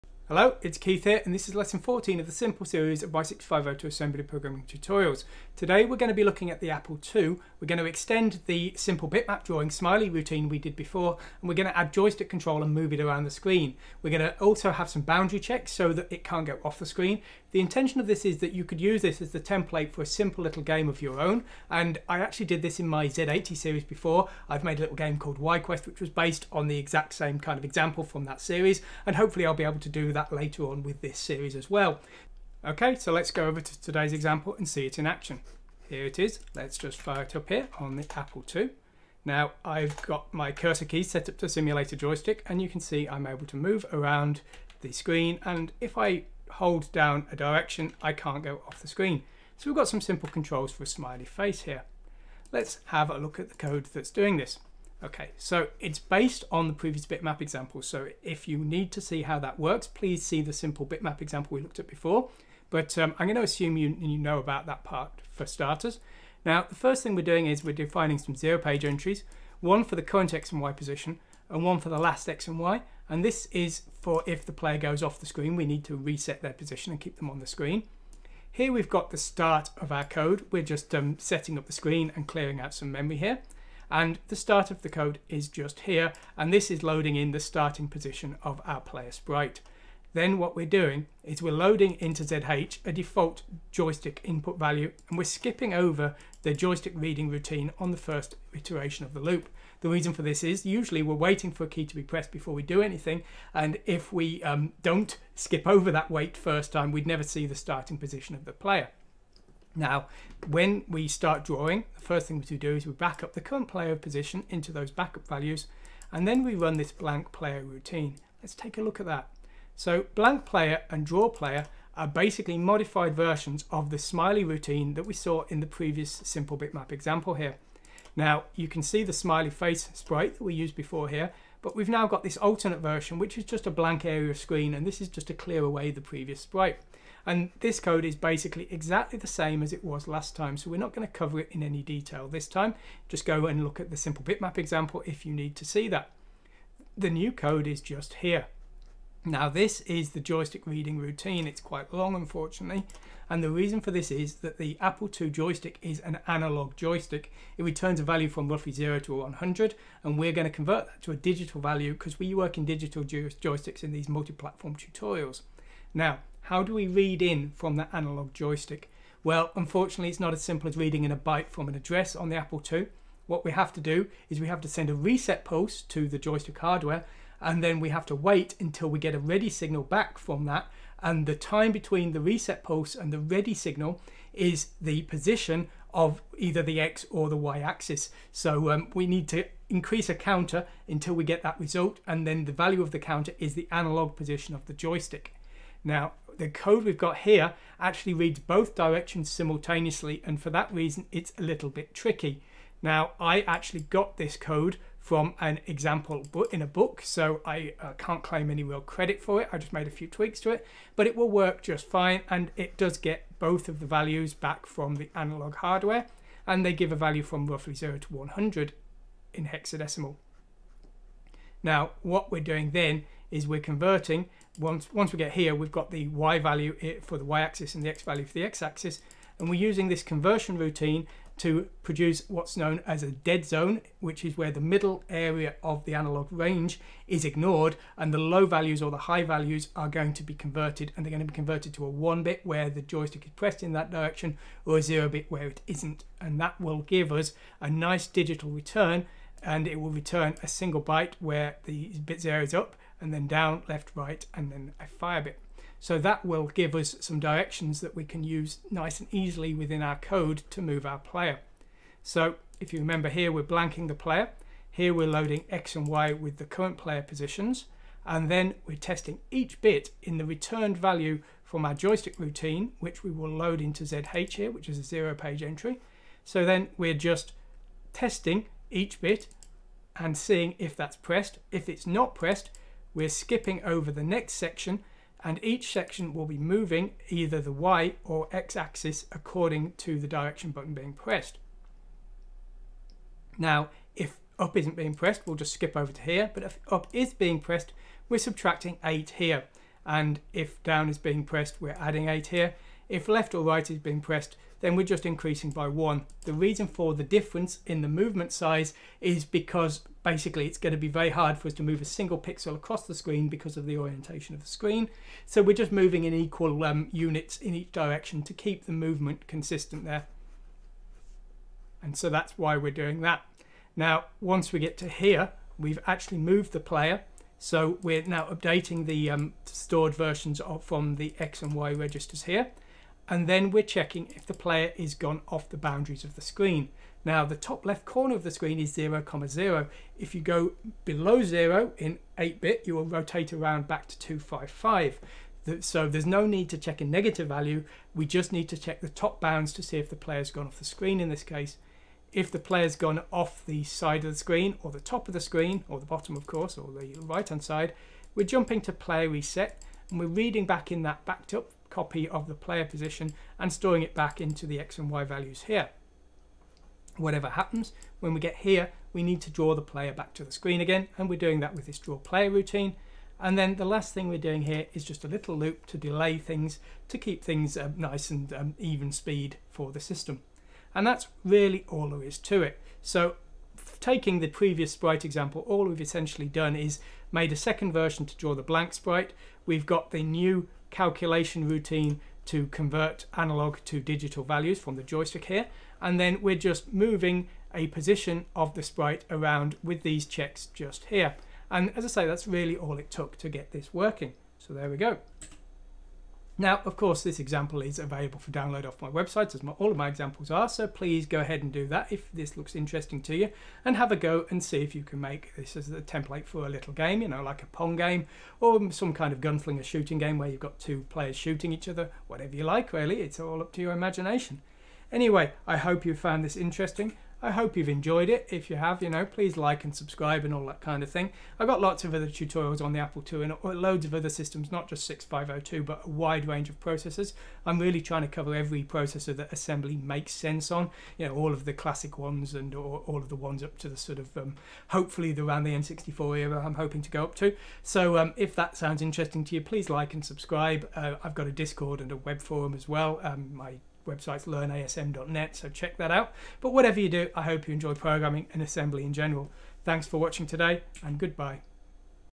Joystick Reading on the Apple II - 6502 Lesson S14
Reading the Apple II Joystick is a serious pain! We have to read a single bit from the ports - and wait until it changes to get the analog values, then convert them to digital. Lets figure it out!

As always, this Video lesson matches the text lesson on my website, and you can get the source code as well...